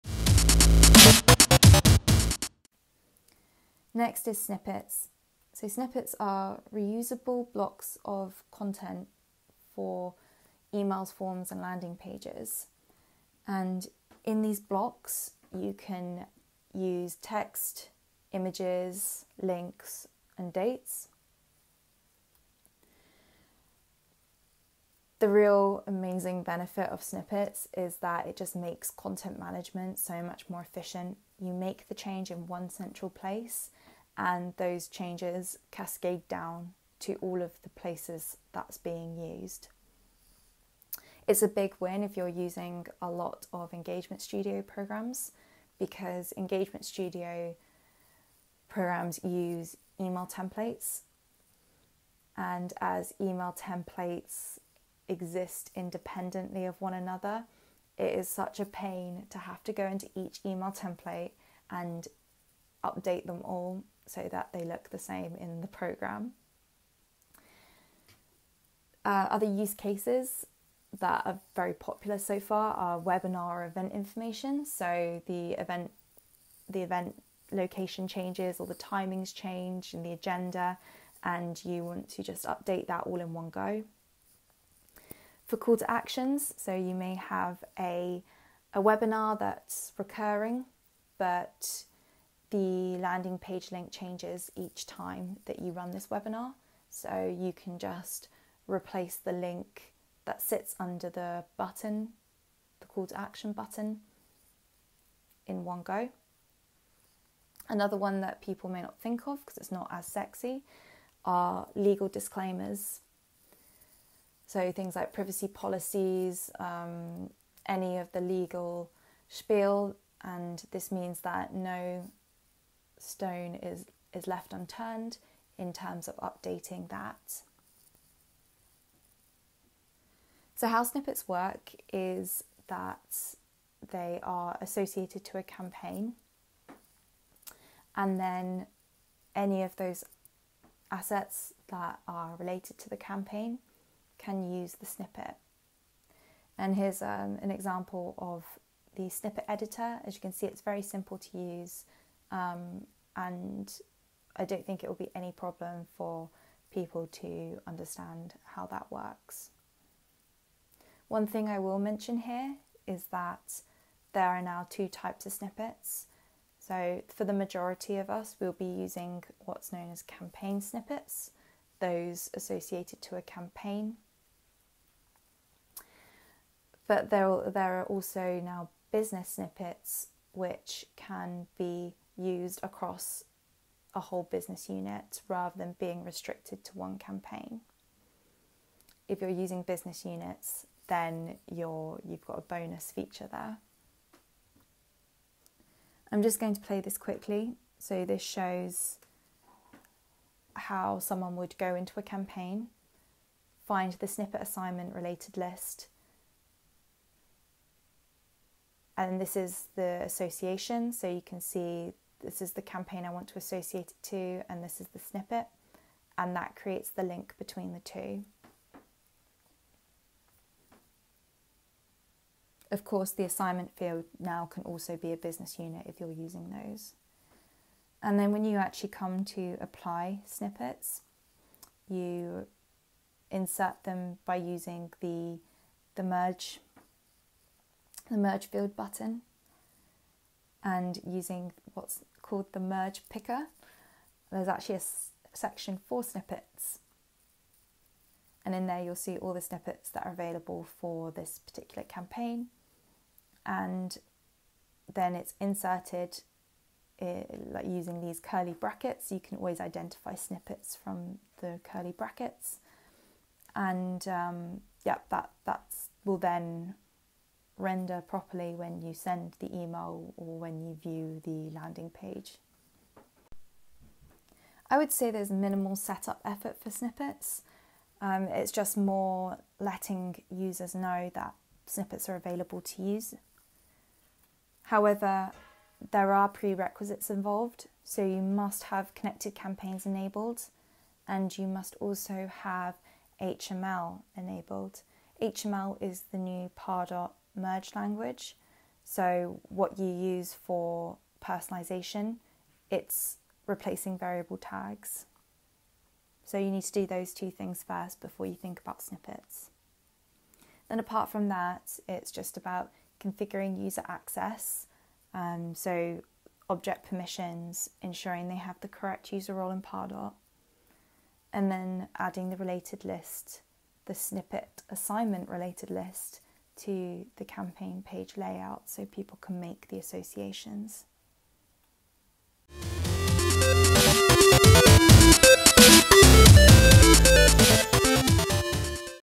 #3 Snippets (5 Pardot Features You May Have Missed)
Snippets are reusable blocks of content for emails, forms & landing pages. Within these blocks, you can use text, images, links, and dates.

The amazing benefit is efficient content management- you make the change in one central place, and those changes cascade down to all of the locations that Snippet is being used. Find out how they work, popular use cases, and how to set up Snippets.

Find Snippets and more on The DRIP:

Introducing Pardot Snippets: A First Look at Reusable Content

This video is part of a series based on my London’s Calling 2020 talk "5 New Pardot Features You May Have Missed”. Look out for the other 4 feature videos in the series!